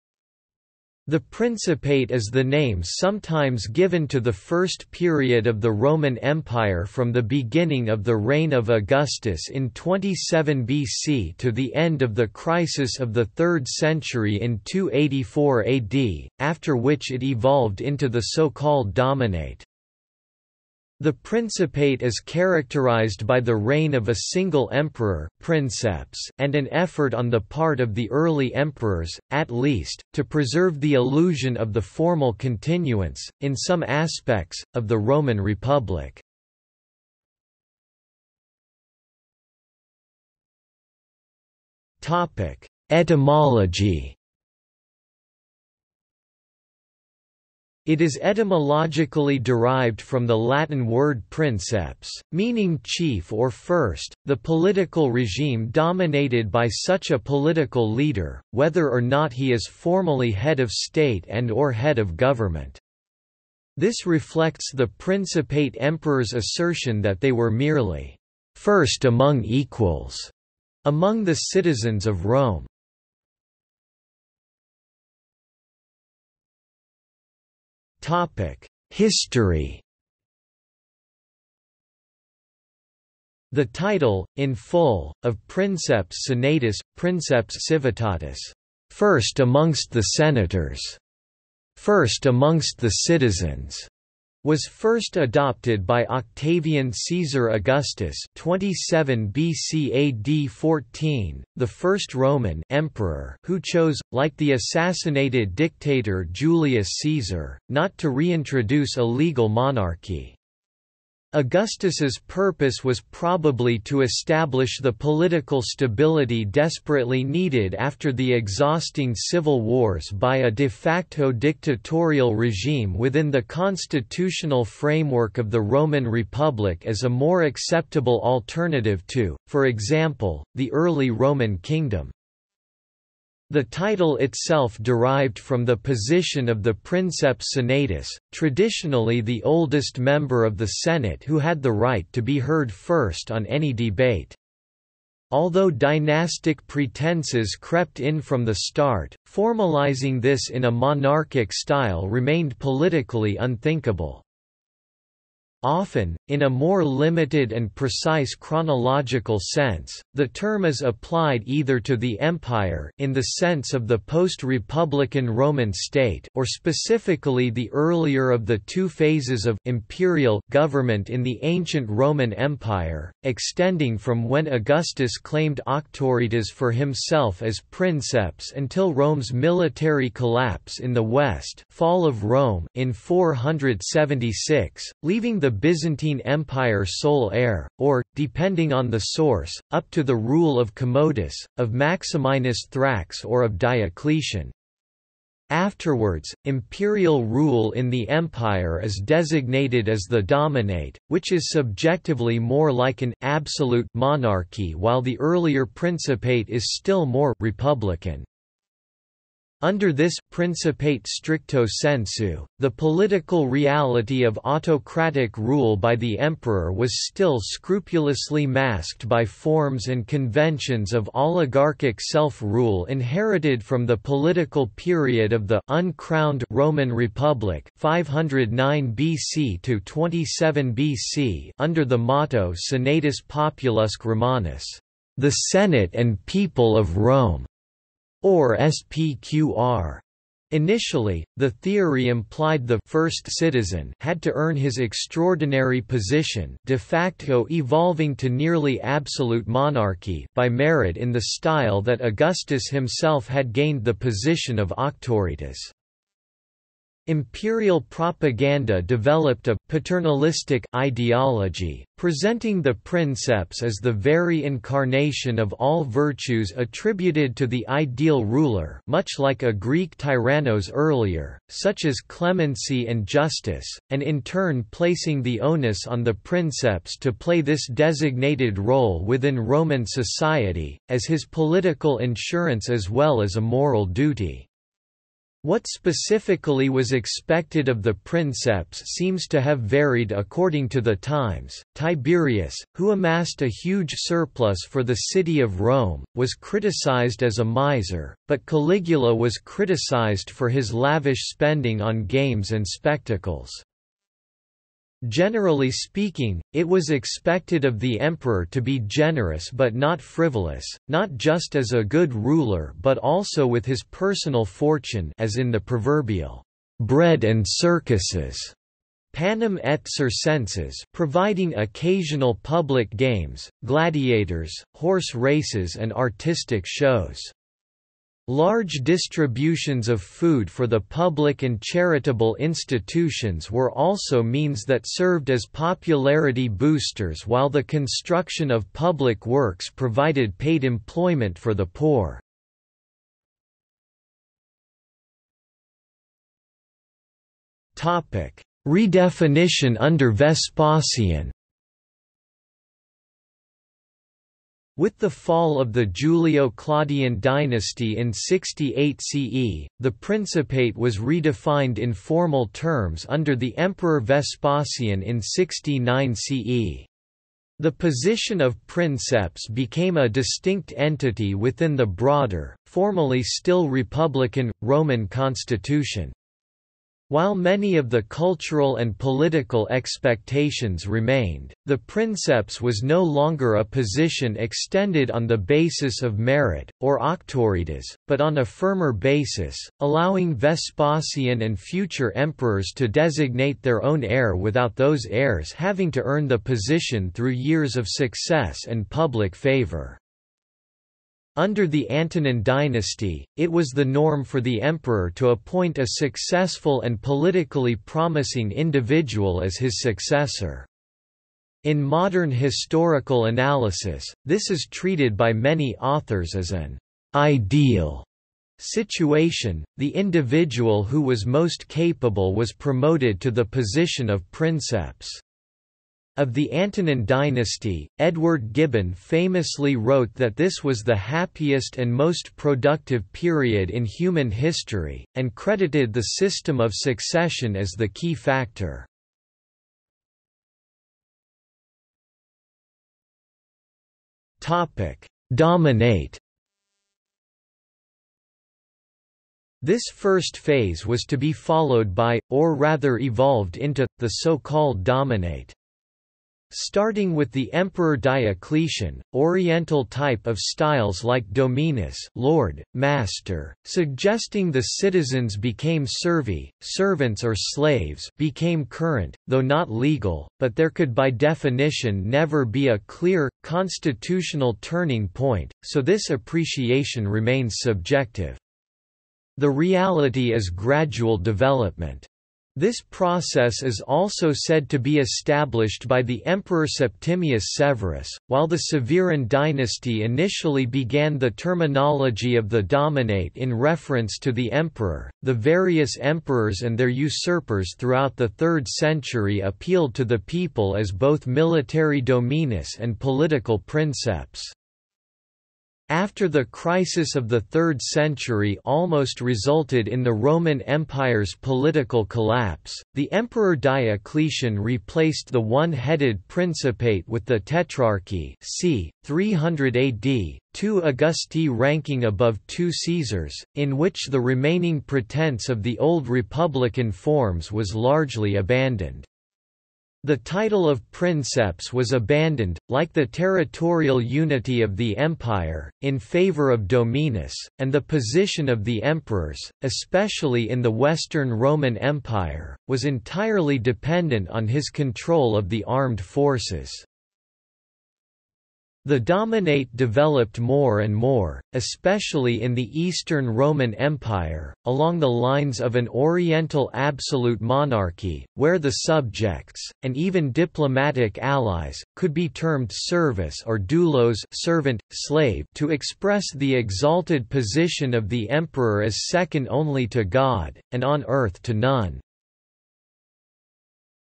Principate | Wikipedia audio article
00:00:38 1 Etymology
00:01:14 2 History
00:05:44 2.1 Redefinition under Vespasian
00:07:21 2.2 iDominate/i

- Socrates

SUMMARY
The Principate is the name sometimes given to the first period of the Roman Empire from the beginning of the reign of Augustus in 27 BC to the end of the Crisis of the Third Century in 284 AD, after which it evolved into the so-called Dominate.
The Principate is characterised by the reign of a single emperor (princeps) and an effort on the part of the early emperors, at least, to preserve the illusion of the formal continuance, in some aspects, of the Roman Republic.